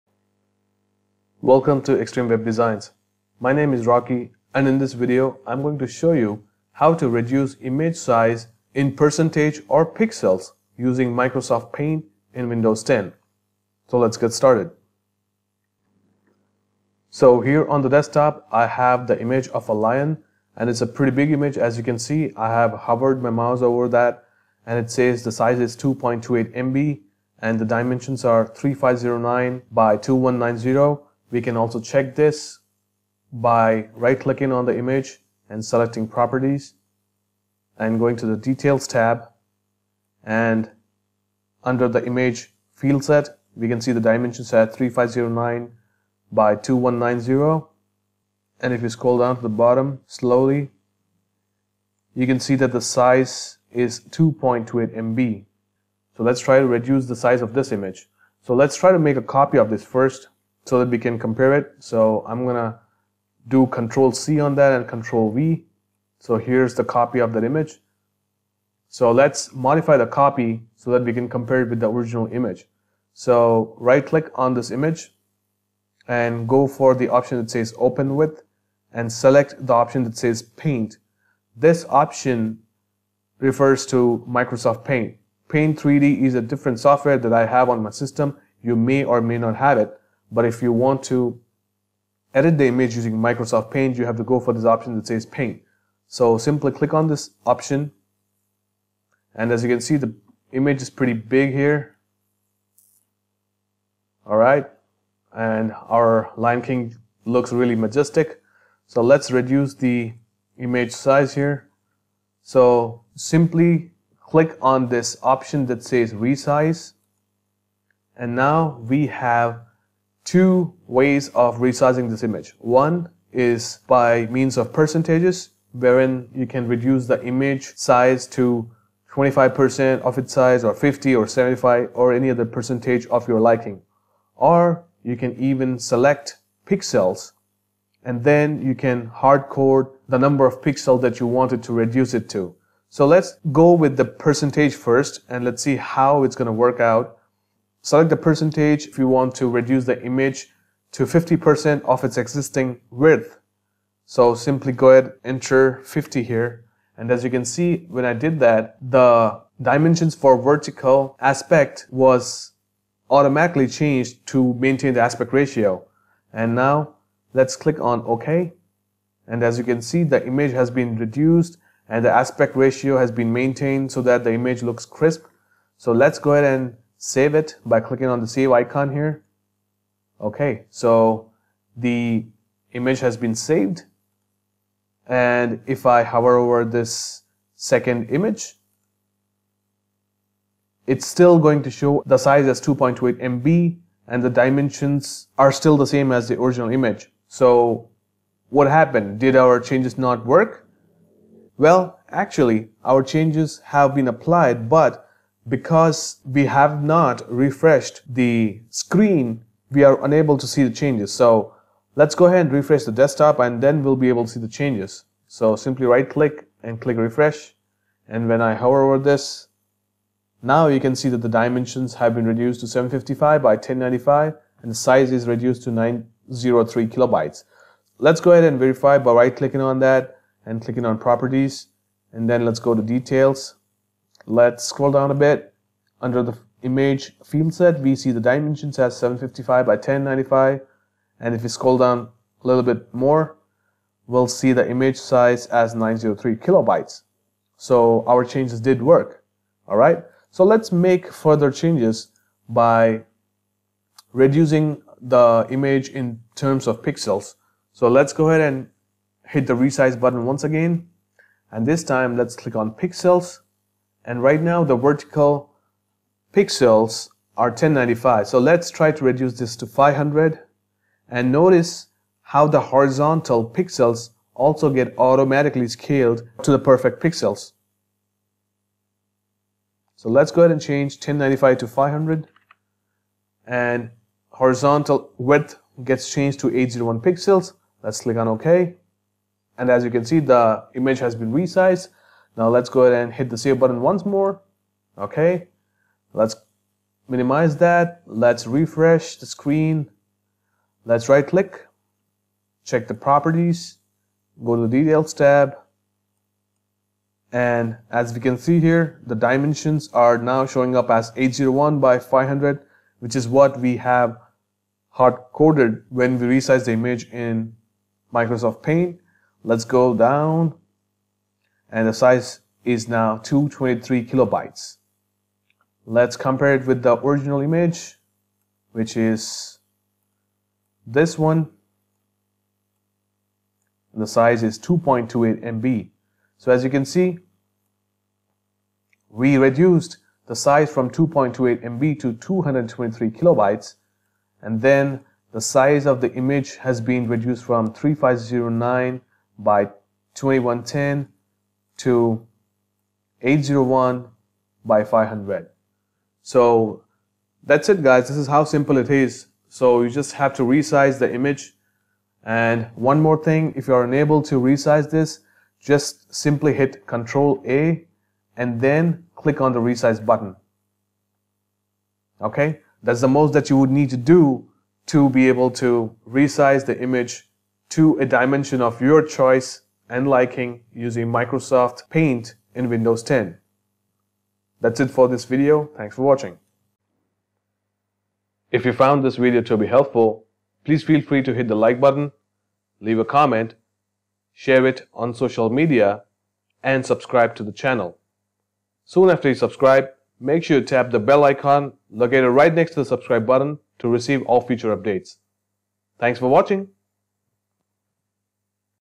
How To Reduce Image Size Using MS Paint In Windows 10
Are you wondering how to reduce image size using MS Paint in Windows 10? Would you like to reduce size of any image from MB to just few KB in few seconds without using Photoshop  and just by using MS Paint? Here is a very easy solution that will let you:
● Resize and image and reduce its size from MB to KB in percentage using MS Paint.
● Resize and image and reduce its size from MB to KB in pixels using MS Paint.
● Maintain aspect ratio to ensure image quality without any loss so that the image appears in high resolution.

Watch all our videos in HD by clicking on the "Change Quality" icon on the YouTube player & select the highest available quality.